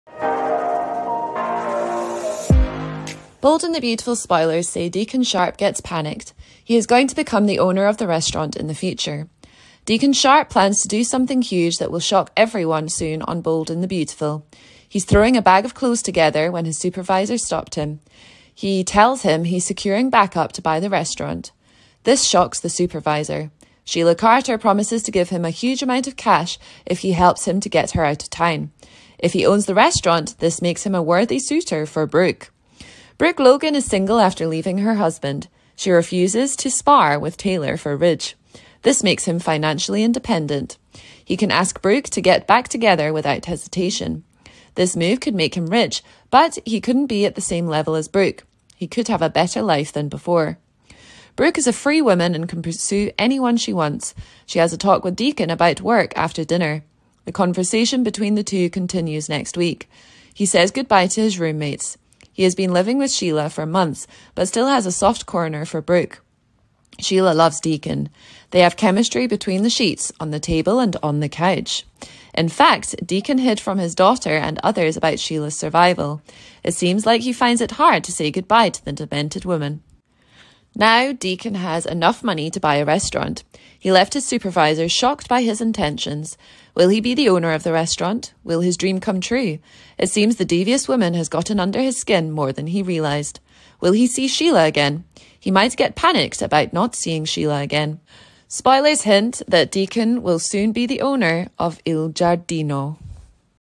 Bold and the Beautiful Spoilers: Deacon's Status Increases After Eatery Buy - Is It All for Brooke
Bold and the Beautiful spoilers say that Deacon Sharpe left his supervisor with his intention. He stuns him that he is going to buy a restaurant. This would make his life better and allow him to become financially independent. He may become a suitor for Brooke Logan, who he always love.